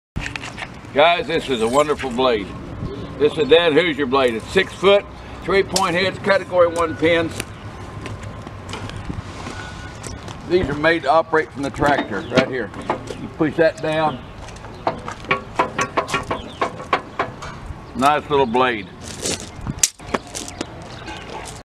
DANUSER CB For Sale
Danuser Grader Blade - 6ft, 3pt Hitch, Category 1 Pins   *See Pics & Video For More Details *Sells Absolute! ~Please Read Terms & Conditions Prior to Bidding*